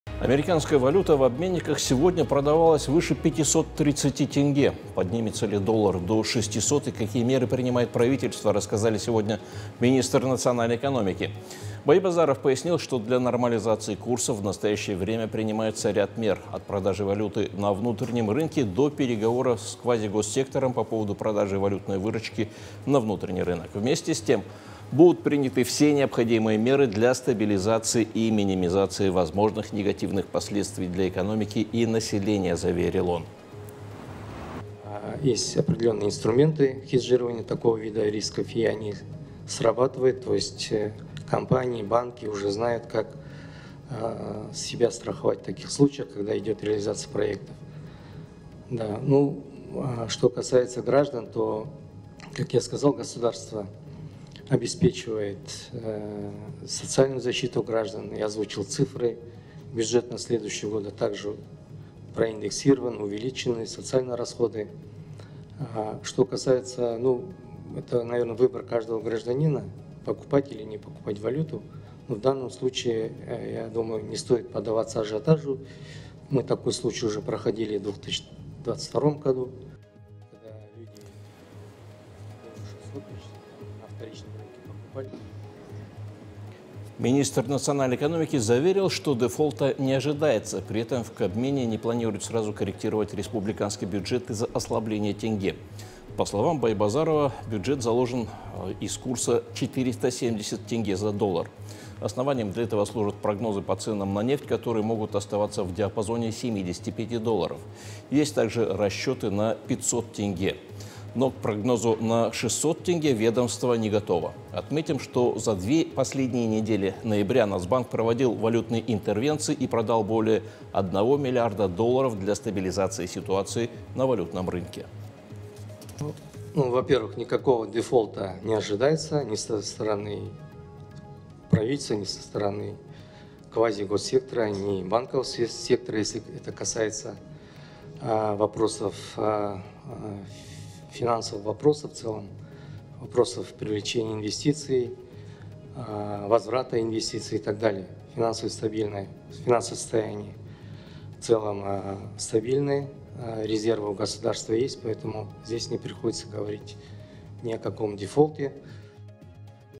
Доллар за 530 тенге: какие меры примет правительство
Нацвалюта в обменниках страны торговалась выше 530 тенге за доллар. Поднимется ли американец до 600 тенге и какие меры принимает правительство, рассказал сегодня министр национальной экономики. Байбазаров пояснил, что для нормализации курса в настоящее время принимается ряд мер – от продажи валюты на внутреннем рынке до переговоров с квазигосударственным сектором по поводу продажи товаров и валютной выручки на внутренний рынок. Вместе с тем будут приняты все необходимые меры для стабилизации и минимизации возможных негативных последствий для экономики и населения, заверил он.